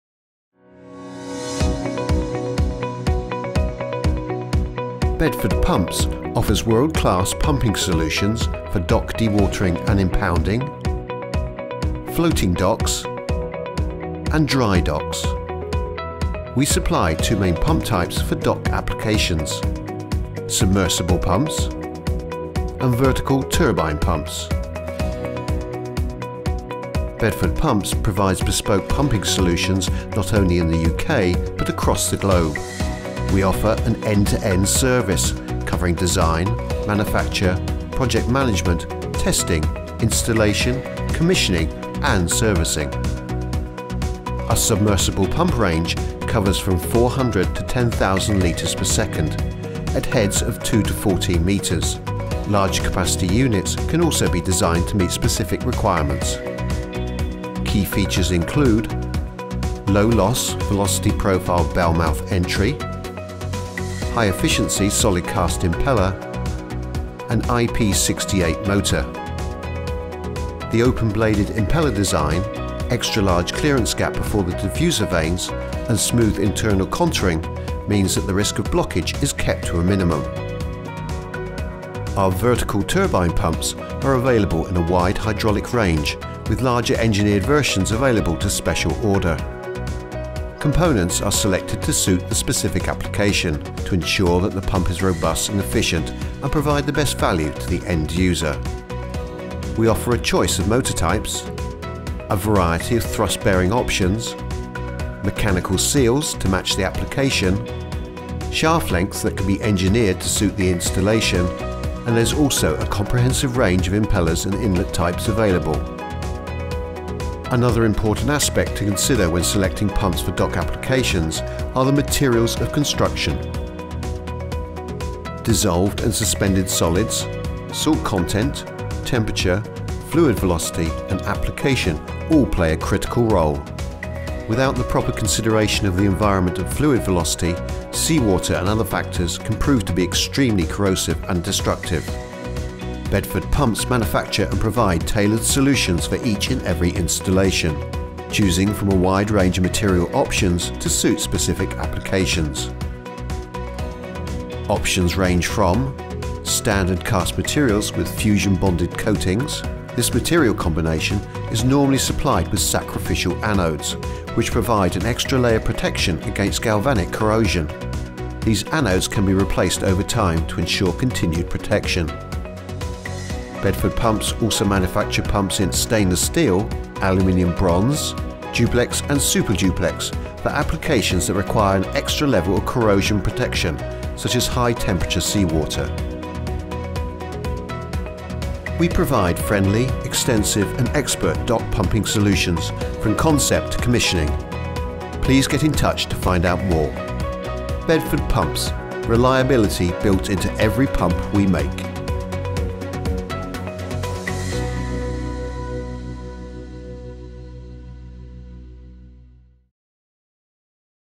Bedford Pumps pumping plant for dock applications
Bedford Pumps provide world class pumping solutions for dock applications including dewatering and impounding, floating docks and dry docks.

From concept through to commissioning Bedford Pumps teams of experts are on hand to provide tailor made solutions to suit specific applications, advising on appropriate materials and components to fit the environment and fluid velocity and ensure the robustness of pumping plant, efficiency and the best value to the end user.